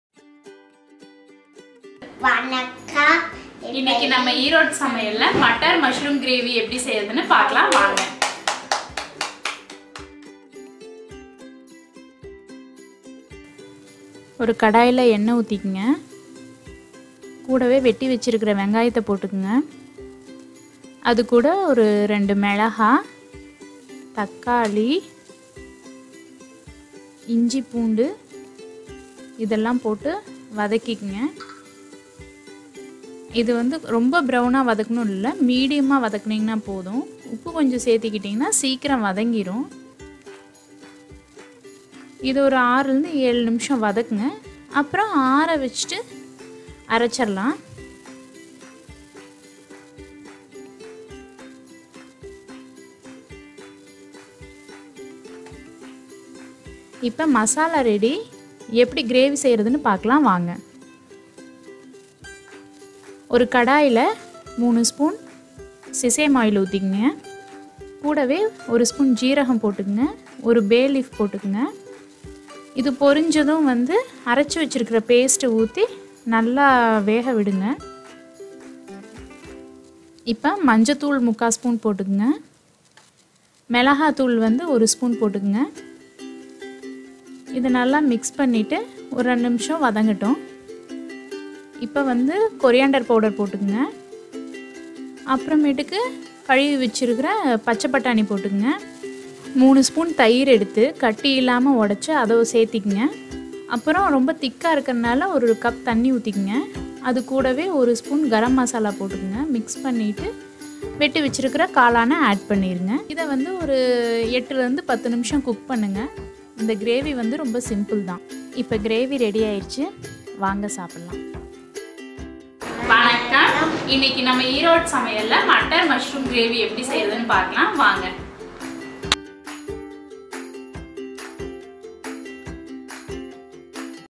மட்டர் மஷ்ரூம் கிரேவி | Matar Mushroom Gravy for chappathi
This is an easy and tasty recipe!! If you get bored with regular sabzi's try this Matar Mushroom Gravy.